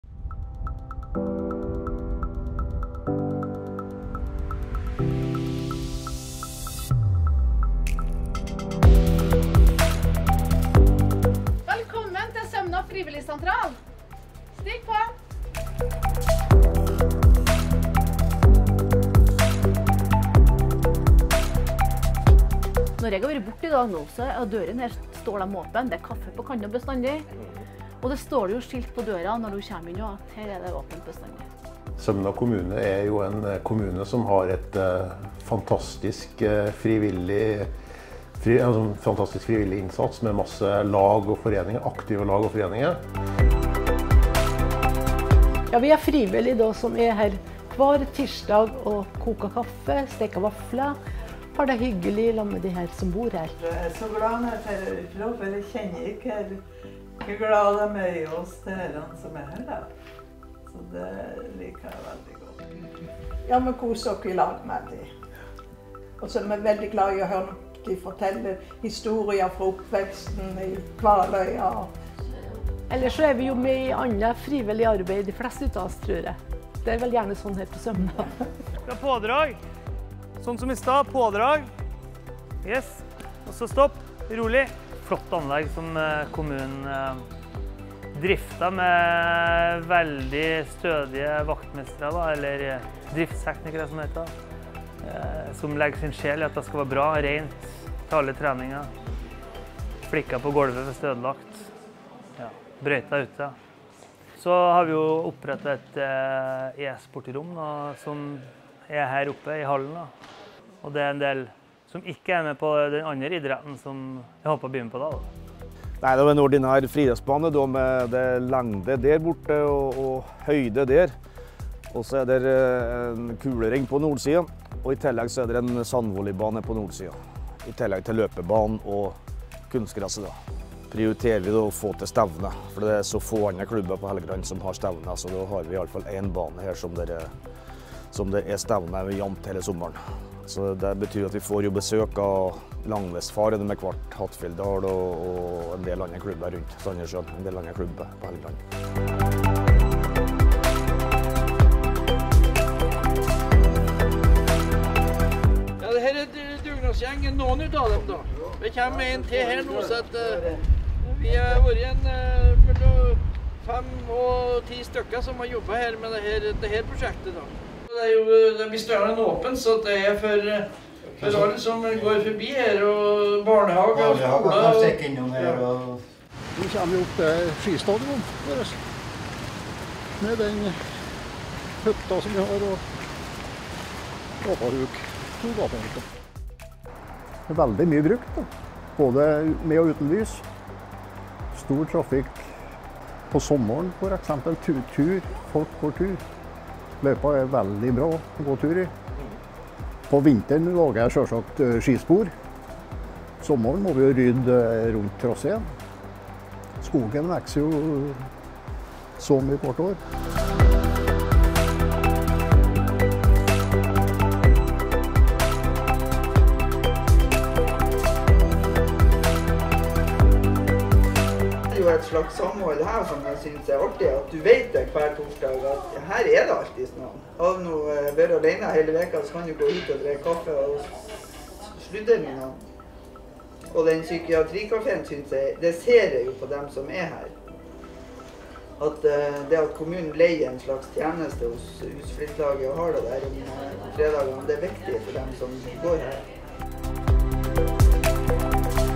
Finalist Årets frivillighetskommune 2021: Sømna kommune
Sømna kommune er finalist til Årets frivillighetskommune 2021. Prisen "Årets frivillighetskommune" ble opprettet av KS og Frivillighet Norge i 2016. Formålet med prisen er å løfte frem kommuner og fylkeskommuner som arbeider godt med frivillighetspolitikk. I utvelgelsen blir det lagt vekt på hvordan kommunen har utviklet politikken i dialog med bredden av frivilligheten, dens politiske forankring, om den legger til rette for faste årlige møteplasser og på hvilken måte man har involvert den lokale frivilligheten i utviklingen av frivillighetspolitikken.